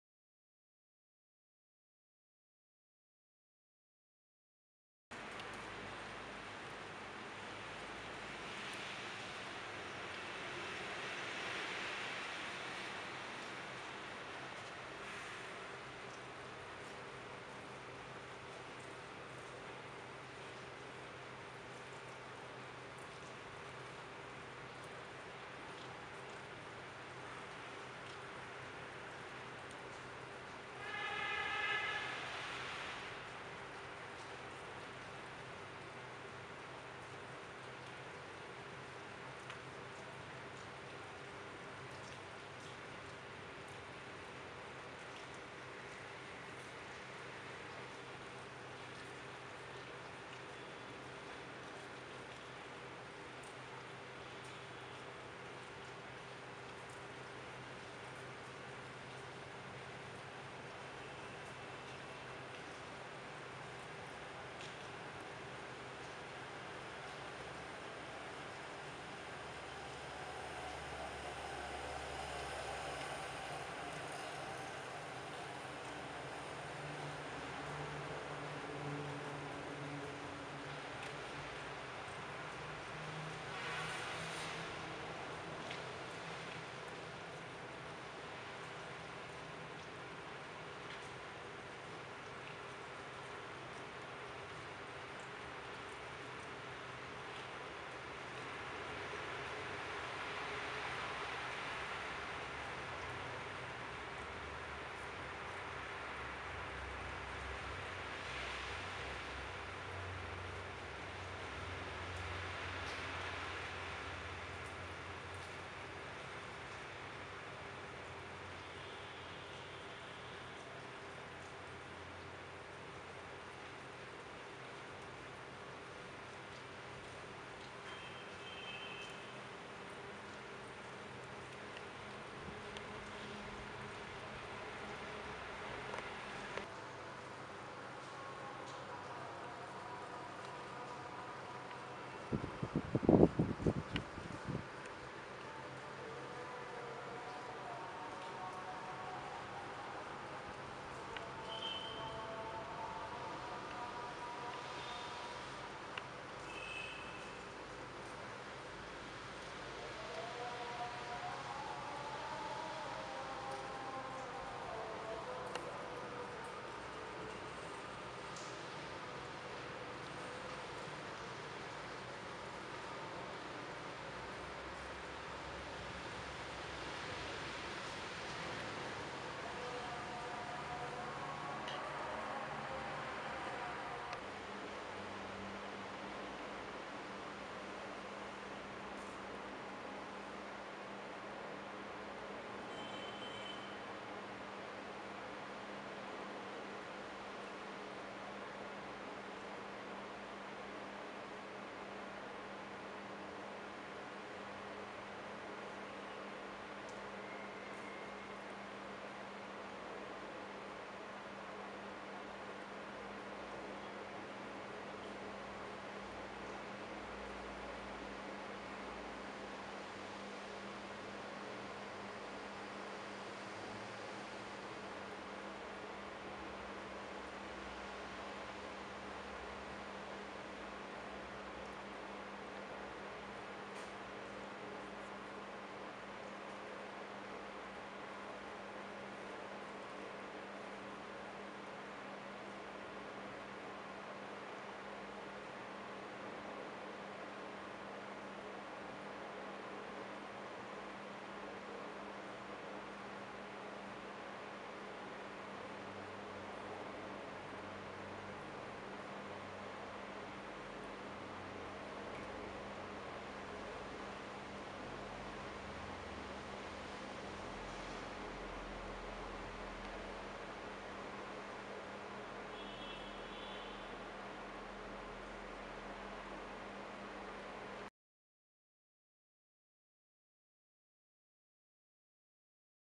Bangalore Rains
The house next to my home had some festival or celebration going on and they had decorated their house with colored light bulbs.

I had to get up for something in the middle of night, must be around 4:00 am and I saw this beautiful reflection of lights in rain water and rain drops falling slowly falling into puddles of water.

I wasted no time and setup my Nikon D3100 at ground level and shot 3 clips. Merged them and posted them here.